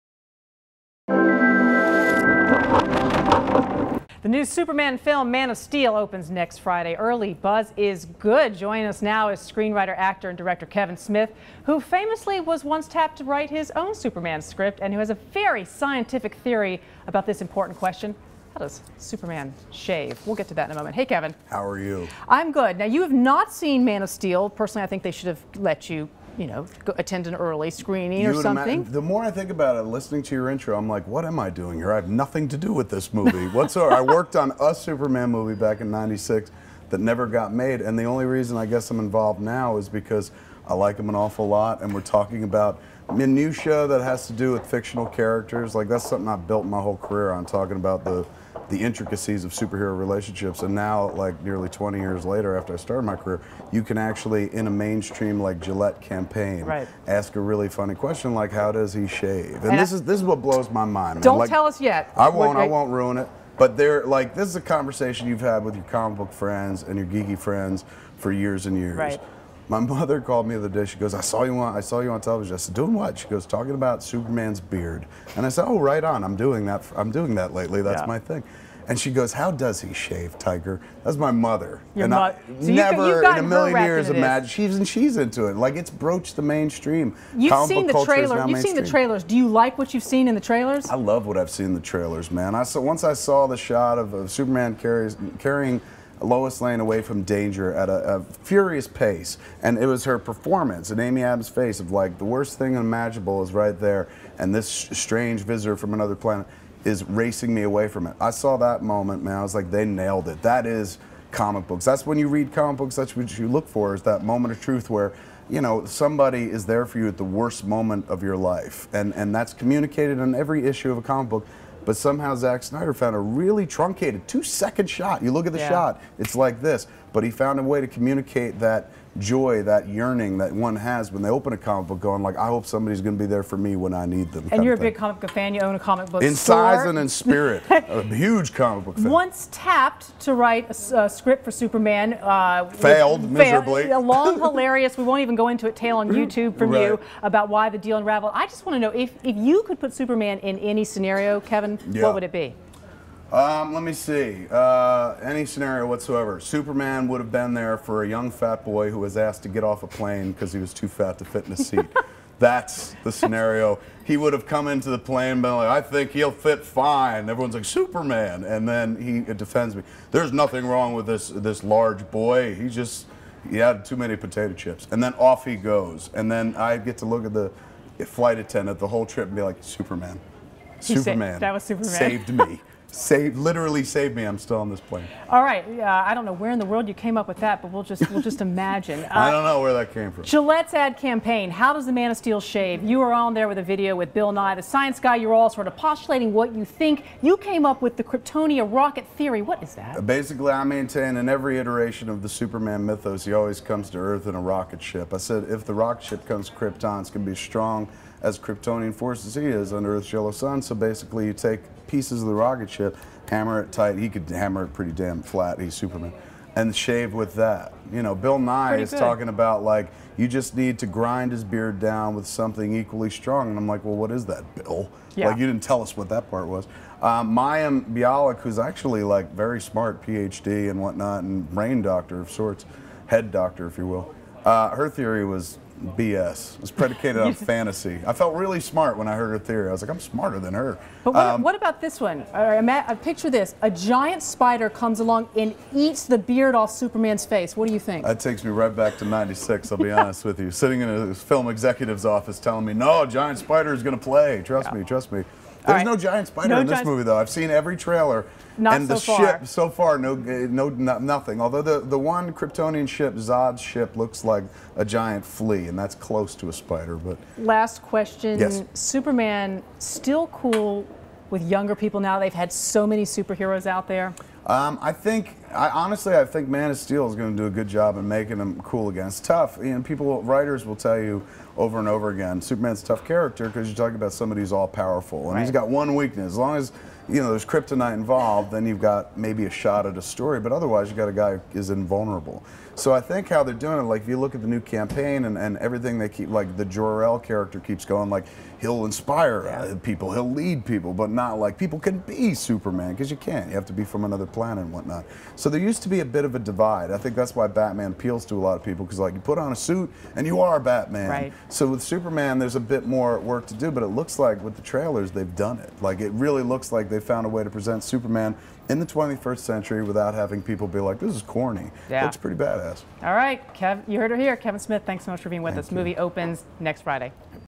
Kevin Smith on Superman Blockbuster 'Man of Steel'
Actor, screenwriter and director Kevin Smith, who was once tapped to write his own Superman script, shares whether he thinks superhero films can sustain their outsized success, and offers his own scientific theory on one important question: How does Superman shave? Photo: AP.